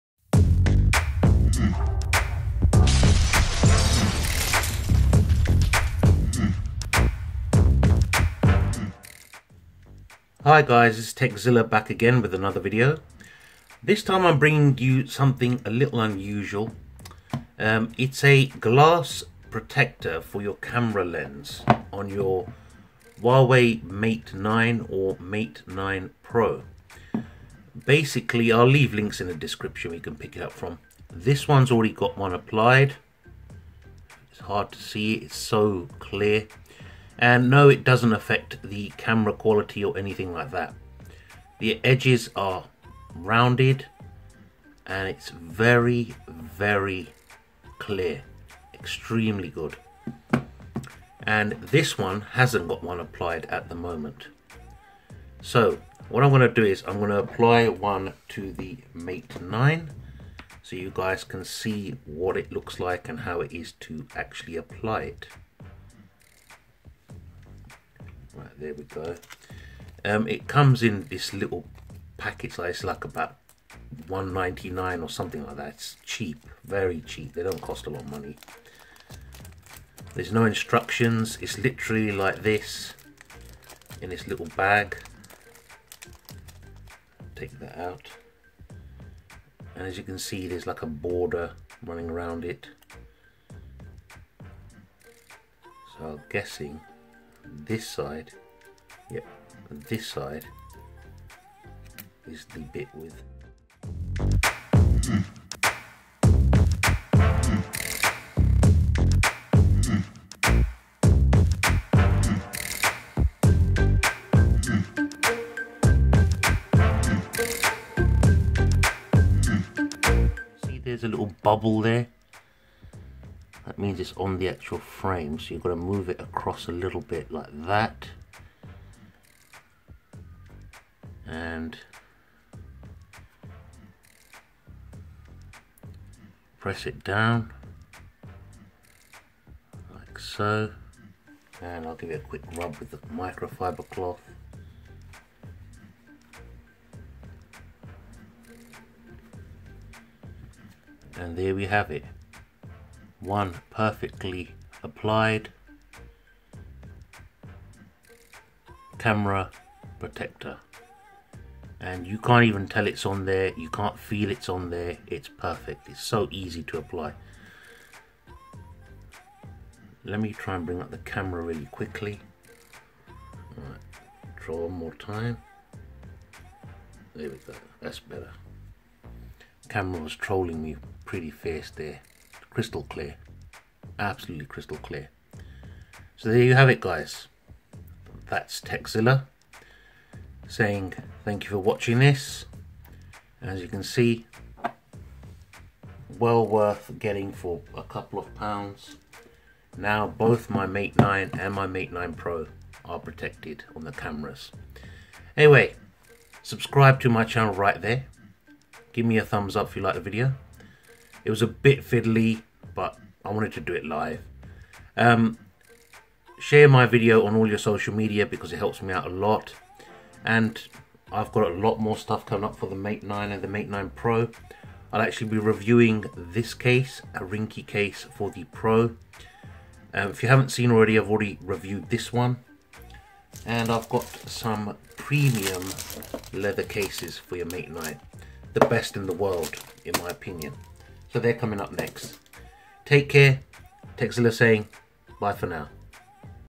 Huawei Mate 9 & Mate 9 Pro - Camera Lens Protector
Do you have a Huawei Mate 9, Mate 9 Pro or the Mate 9 Porsche Design? Then you have the Leica Dual Camera Lens set up on the back of your smartphone and it can be scratched and broken very easily. Watch this Video as i show you how to make your Lens is protected and how to prevent scratches and breakage for £2.99!
This is a Must Watch, so what are you waiting for?

EBay UK
EBay France
EBay Germany
EBay Italy
EBay USA
EBay Canada
EBay Australia